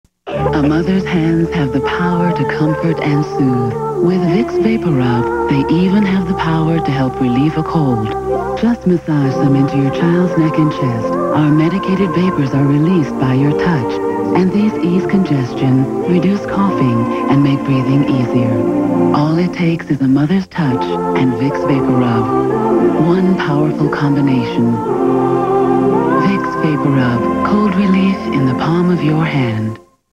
Vics Vaporub Cream commercial from 1996
Pulled from December 22nd 1996 broadcast of VIPER on UPN38 WSBK